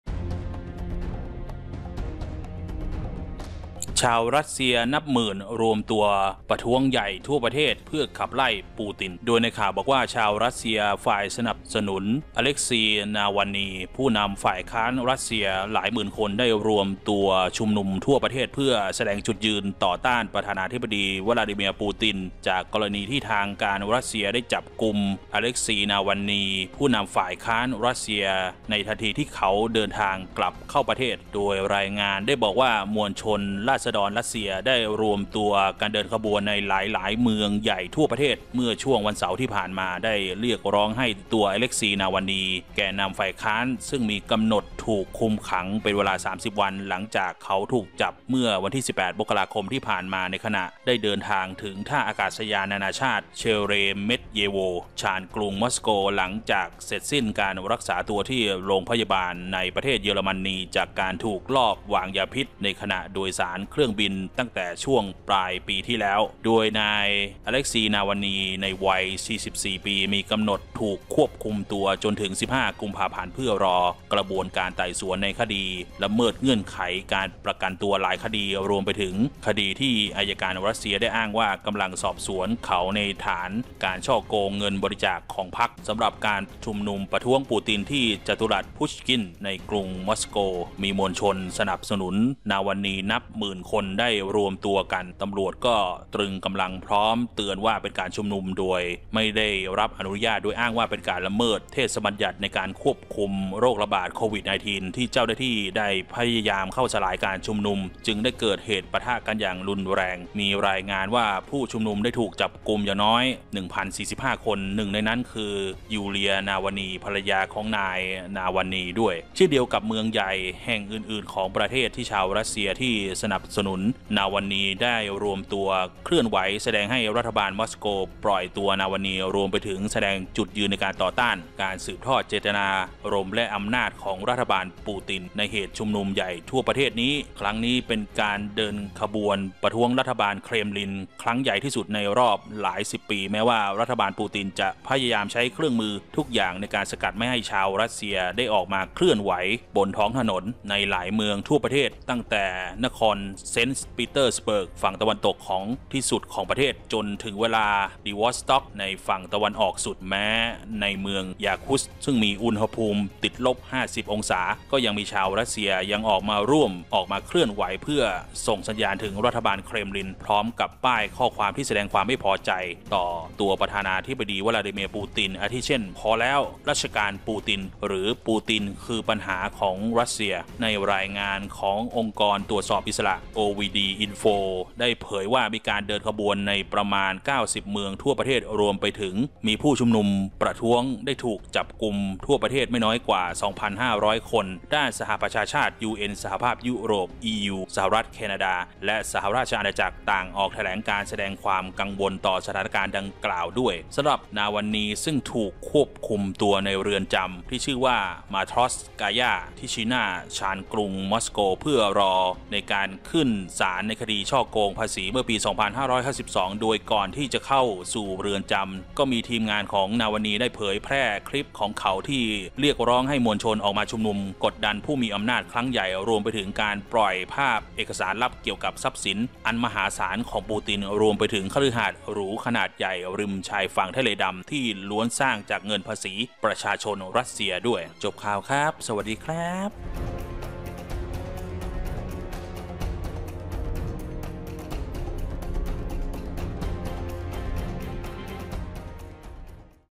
ปูตินงานเข้า! รัสเซียวุ่นหนัก!ม็อบหนุน-ต้าน‘ปูติน’นับแสนชุมนุมทั่วประเทศ
24 ม.ค.64 สำนักข่าวต่างประเทศรายงานว่า ชาวรัสเซียฝ่ายสนับสนุน "อเล็กซี นาวัลนี" ผู้นำฝ่ายค้านรัสเซีย หลายหมื่นคนรวมตัวชุมนุมทั่วประเทศ เพื่อแสดงจุดยืนต่อต้านประธานาธิบดีวลาดิเมียร์ ปูติน จากกรณีที่ทางการรัสเซียจับกุม นาวัลนี ผู้นำฝ่ายค้านรัสเซียในทันทีที่เขาเดินทางกลับเข้าประเทศ

รายงานระบุว่า มวลชนราษฎรรัสเซียรวมตัวเดินขบวนในหลายเมืองใหญ่ทั่วประเทศเมื่อช่วงวันเสาร์ที่ผ่านมา เรียกร้องให้ตัวอเล็กซี นาวัลนี แกนนำฝ่ายค้านซึ่งมีกำหนดถูกคุมขังเป็นเวลา 30 วัน หลังเขาถูกจับเมื่อ 18 ม.ค. ที่ผ่านมา ขณะเดินถึงท่าอากาศยานนานาชาติเชเรเมตเยโว ชานกรุงมอสโก หลังเสร็จสิ้นการรักษาตัวที่โรงพยาบาลในประเทศเยอรมนีจากการถูกลอบวางยาพิษขณะโดยสารเครื่องบินตั้งแต่ช่วงปลายปีที่แล้ว

นายนาวัลนี เรียกร้องให้ผู้สนับสนุนเขาออกมาประท้วง หลังจากที่เขาถูกจับกุมเมื่อสุดสัปดาห์ที่แล้ว ทันทีที่เขาเดินทางกลับจากเยอรมนีมายังกรุงมอสโกเป็นครั้งแรก หลังจากที่ถูกวางยาพิษเมื่อเดือนสิงหาคมและเดินทางไปพักรักษาตัวในเยอรมนี การประท้วงต่อต้านนายปูตินครั้งนี้เกิดขึ้นในหลายเมืองทั้งในเขตตะวันออกไกลและไซบีเรีย ท่ามกลางอากาศที่เย็นจัด ที่กรุงมอสโก นครหลวงของรัสเซีย

ผู้ประท้วงวางแผนที่จะไปรวมตัวกันที่จัตุรัสพุชกิ้น ในเวลา 14.00 น. หรือ ตรงกับเวลา 18.00 น. ตามเวลาในประเทศไทย ก่อนที่จะเดินขบวนไปยังพระราชวังเครมลิน ซึ่งเป็นทำเนียบรัฐบาลรัสเซีย หน่วยงานที่ดูและสื่อของรัสเซียเตือนบรรดาแพลตฟอร์มออนไลน์ทั้งหลายมิให้เชิญชวนให้เยาวชนเข้าร่วมในการชุมนุม มิฉะนั้นจะโดนปรับอย่างรุนแรง โดยทางการรัสเซียกล่าวว่า แพลทฟอร์ม อย่างติ๊กต่อกและยูทูป เริ่มลบเนื้อหาตามที่ทางการขอร้องแล้ว นายนาวัลนี อดีตทนายความ ซึ่งกล่าวหาประธานาธิบดีวลาดิเมียร์ ปูติน ว่าเป็นผู้ออกคำสั่งให้ฆาตกรรมเขา อาจจะต้องอยู่ในเรือนจำเป็นเวลายาวนานในคดีที่เขาระบุว่าถูกสร้างขึ้นมา ในขณะที่นายปูตินปฎิเสธว่า ไม่มีส่วนเกี่ยวข้องใดๆ นายนาวัลนี เป็นผู้ที่วิพากษ์วิจารณ์รัฐบาลรัสเซียในเรื่องทุจริตและโกงการเลือกตั้ง